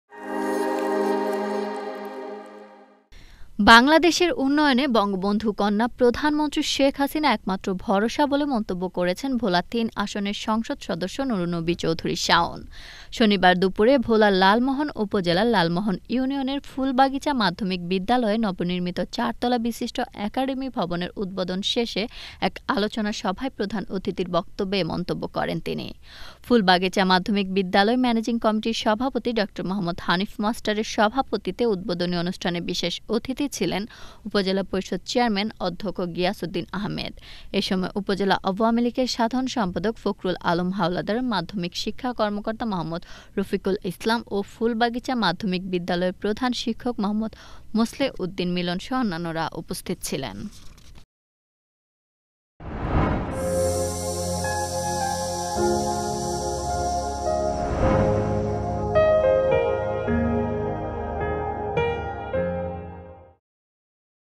লালমোহনে ফুলবাগিচা মাধ্যমিক বিদ্যালয়ের একাডেমিক ভবনের উদ্বোধন | Channel S News | Channel S 2023
বাংলাদেশের উন্নয়নে বঙ্গবন্ধু কন্যা প্রধানমন্ত্রী শেখ হাসিনা একমাত্র ভরসা বলে মন্তব্য করেছেন ভোলা-৩ আসনের সংসদ সদস্য নূরুন্নবী চৌধুরী শাওন.

1: News :
2: Entertainment News :
3: Sports News :